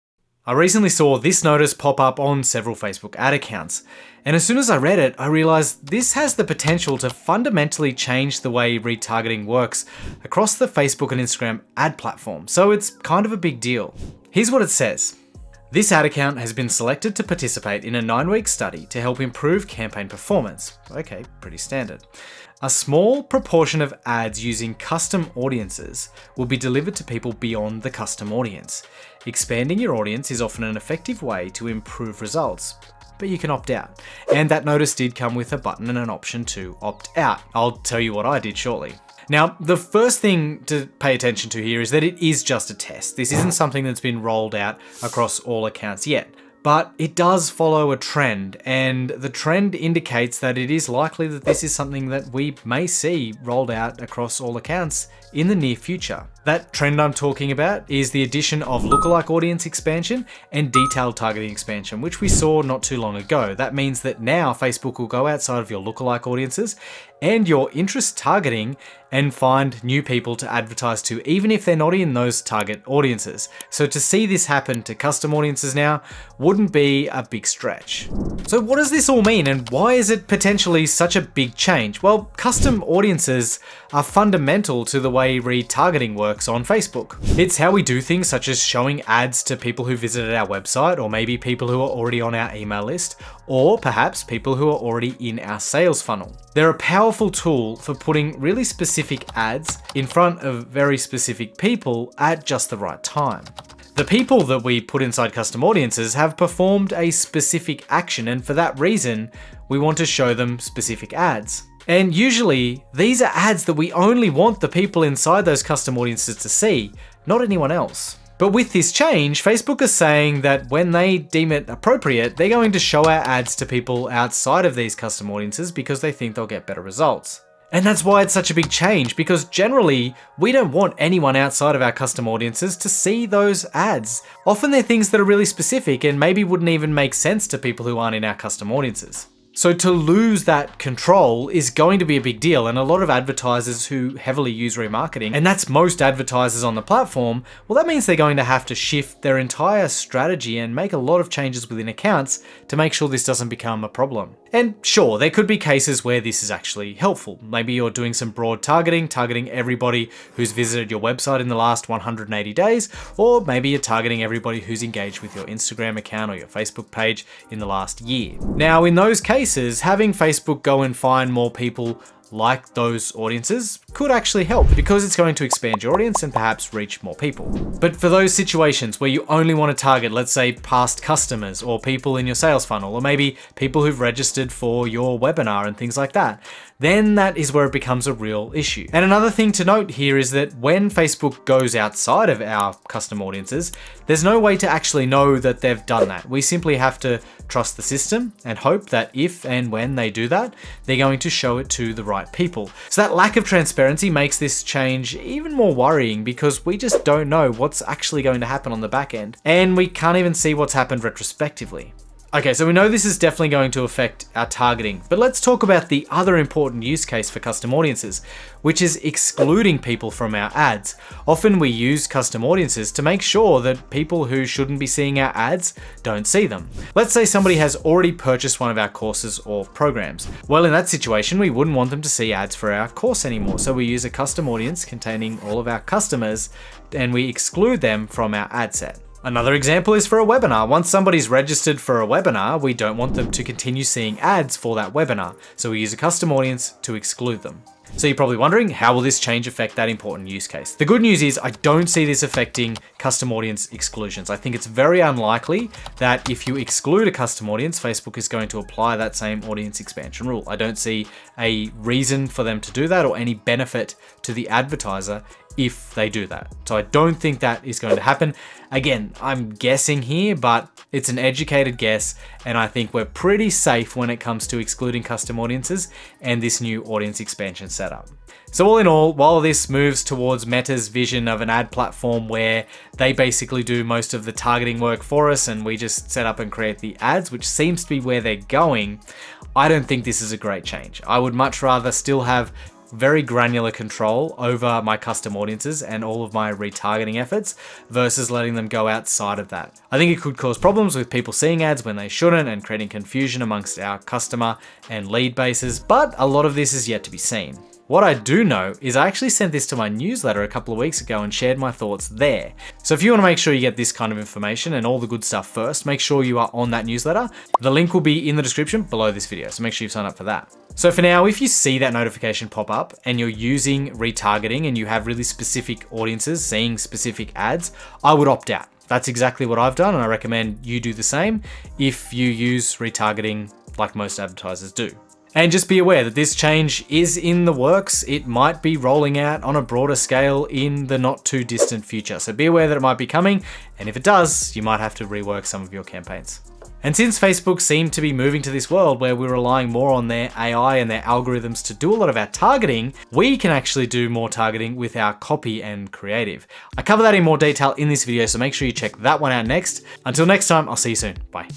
This Change Could End Retargeting As We Know It
Facebook retargeting is still one of the most effective advertising strategies on the platform. But one big change that's currently being tested by Facebook could force us to completely rethink the way we do it.

We've seen the rollout of detailed targeting expansion and lookalike audience expansion. Both of which promise to deliver better results by allowing Facebook to target outside our target audiences. But this change to Custom Audiences is a completely different ball game.

The implications of this change mean Facebook advertisers will need to re-think they way we set up and structure our remarketing campaigns.

💎 Join the best newsletter for course creators and coaches who want to generate more sales using Facebook ads, evergreen sales funnels and conversion rate optimization